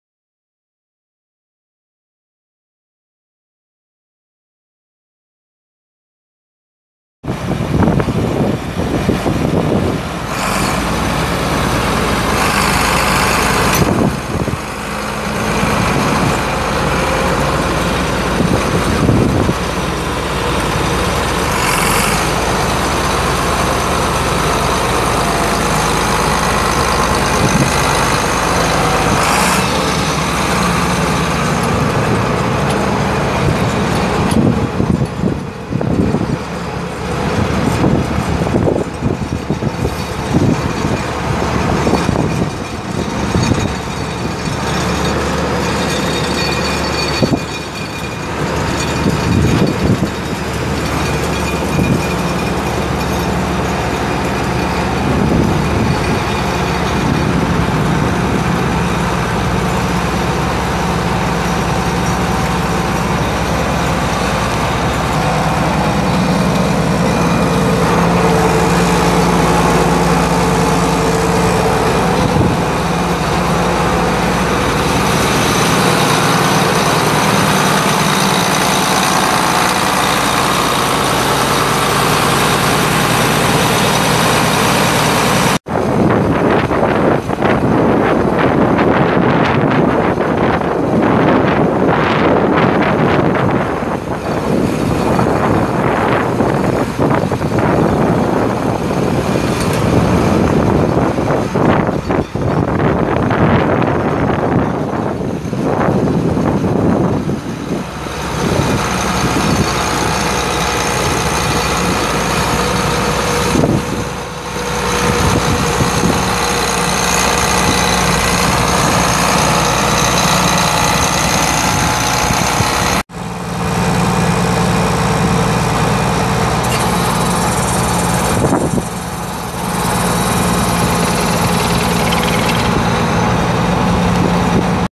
1982 John Deere 8650 4WD tractor for sale | no-reserve Internet auction March 8, 2017
View a detailed listing of this item and place bids

1982 John Deere 8650 4WD tractor
212 hours on meter
Approx. 10,212 total hours
16F-6R gears
Enclosed cab
AC and heat
AM/FM
Work lights
Three point
Quick attach
1000 PTO
(3) rear hydraulic remote sets
214 gallon fuel tank
125" wheelbase
18.4-38 tires
Serial RW8650H001752
It's located in Partridge, KS, and we're selling it to the highest bidder regardless of price.